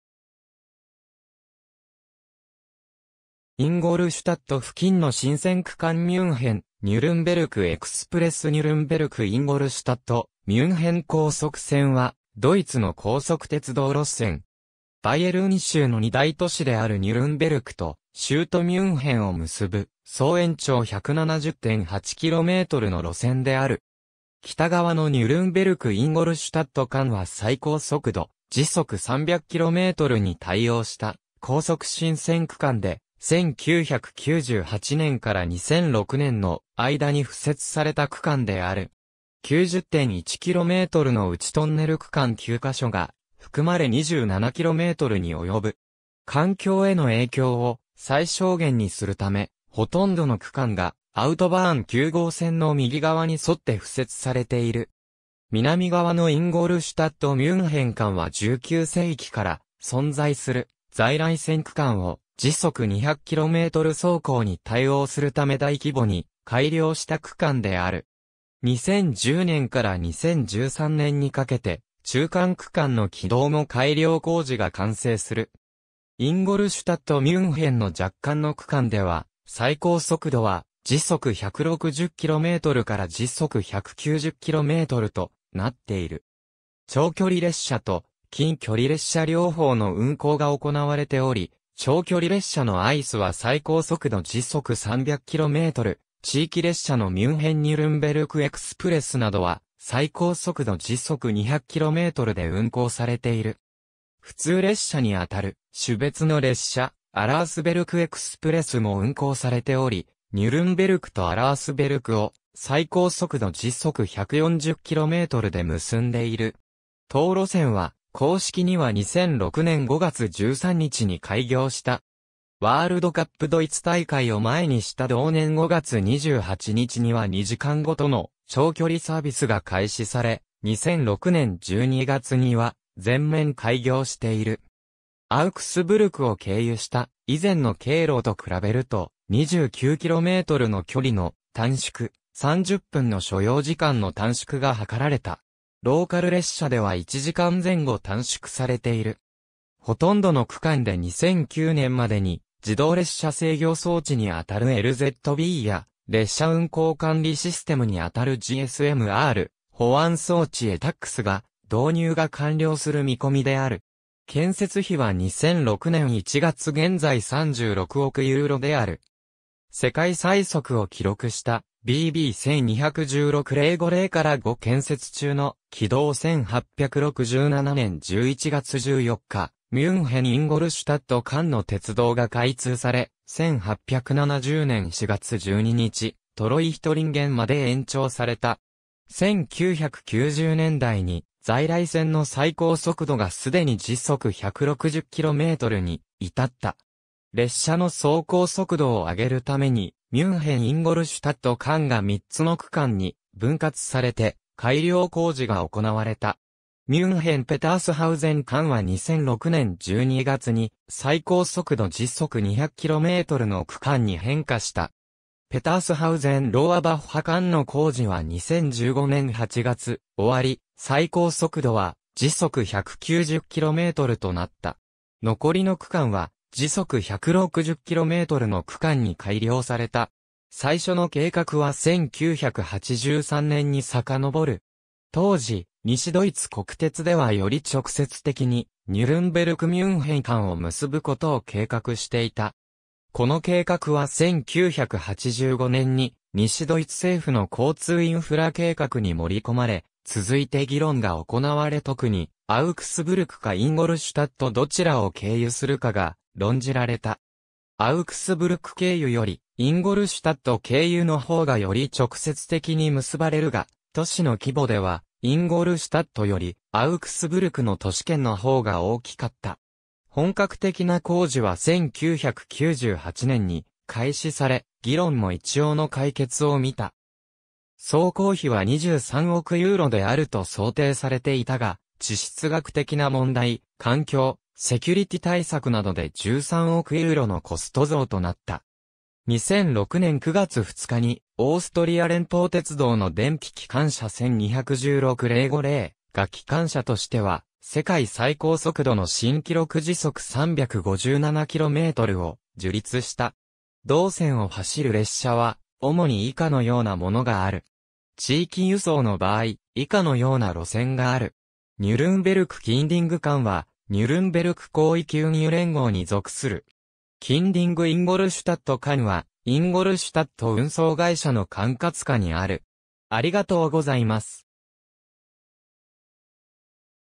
ニュルンベルク-インゴルシュタット-ミュンヘン高速線
インゴルシュタット付近の新線区間 ミュンヘン-ニュルンベルク-エクスプレス ニュルンベルク-インゴルシュタット-ミュンヘン高速線 (ドイツ語: Schnellfahrstrecke Nürnberg–Ingolstadt–München) はドイツの高速鉄道路線。
バイエルン州の二大都市であるニュルンベルクと州都ミュンヘンを結ぶ、総延長170.8kmの路線である。
北側のニュルンベルク・インゴルシュタット間は最高速度300km/hに対応した高速新線区間 (NBS) で、1998年から2006年の間に敷設された区間である。
90.1kmのうちトンネル区間9ヶ所が含まれ27kmにおよぶ。
環境への影響を最小限にするため、ほとんどの区間がアウトバーン9号線の右側に沿って敷設されている。
南側のインゴルシュタット・ミュンヘン間は19世紀から存在する在来線区間を200km/h走行に対応するため大規模に改良した区間 (ABS) である。
2010年から2013年にかけて中間区間の軌道も改良工事が完成する。
インゴルシュタット・ミュンヘンの若干の区間では最高速度は160km/hから190km/hとなっている。
長距離列車と近距離列車両方の運行が行われており、長距離列車のICEは最高速度300km/h、地域列車のミュンヘン・ニュルンベルク・エクスプレスなどは最高速度200km/hで運行されている。
普通列車にあたる種別の列車アラースベルクエクスプレスも運行されておりニュルンベルクとアラースベルクを最高速度140km/hで結んでいる。
当路線は、公式には2006年5月13日に開業した。
ワールドカップドイツ大会を前にした同年5月28日には2時間毎の長距離サービスが開始され、2006年12月には全面開業している。
アウクスブルクを経由した以前の経路と比べると、29kmの距離の短縮、30分の所要時間の短縮が図られた。
ローカル列車（ミュンヘン-ニュルンベルク-エクスプレス）では1時間前後短縮されている。
ほとんどの区間で2009年までに自動列車制御装置にあたるLZBや列車運行管理システムにあたるGSM-R、保安装置ETCSが導入が完了する見込みである。
建設費は2006年1月現在36億ユーロである。
世界最速を記録したÖBB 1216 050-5 建設中の軌道 1867年11月14日ミュンヘン - インゴルシュタット間の鉄道が開通され、1870年4月12日トロイヒトリンゲンまで延長された。
1990年代に在来線の最高速度が既に160 km/hに至った。
列車の走行速度を上げるために、ミュンヘン - インゴルシュタット間が三つの区間に分割されて改良工事が行われた。
ミュンヘン - ペタースハウゼン間は2006年12月に最高速度200 km/hの区間に変化した。
ペタースハウゼン - ローアバッハ間の工事は2015年8月終わり、最高速度は190 km/hとなった。
残りの区間は160 km/hの区間に改良された。
最初の計画は1983年にさかのぼる。
当時、西ドイツ国鉄ではより直接的にニュルンベルク・ミュンヘン間を結ぶことを計画していた。
この計画は1985年に西ドイツ政府の交通インフラ計画に盛り込まれ、続いて議論が行われ特にアウクスブルクかインゴルシュタットどちらを経由するかが論じられた。
アウクスブルク経由(199km)よりインゴルシュタット経由(171km)の方がより直接的に結ばれるが、都市の規模ではインゴルシュタットよりアウクスブルクの都市圏の方が大きかった。
本格的な工事は1998年に開始され、議論も一応の解決をみた。
総工費は23億ユーロであると想定されていたが、地質学的な問題、環境、セキュリティ対策などで13億ユーロのコスト増となった。
2006年9月2日にオーストリア連邦鉄道の電気機関車1216 050が機関車としては世界最高速度の新記録357km/hを樹立した。
同線を走る列車は、主に以下のようなものがある。
地域輸送の場合、以下のような路線がある。
ニュルンベルク - キンディング間はニュルンベルク広域運輸連合（Verkehrsverbund Großraum Nürnberg、VGN）に属する。
キンディング - インゴルシュタット間はインゴルシュタット運送会社（Ingolstädter Verkehrsgesellschaft、INVG）の管轄...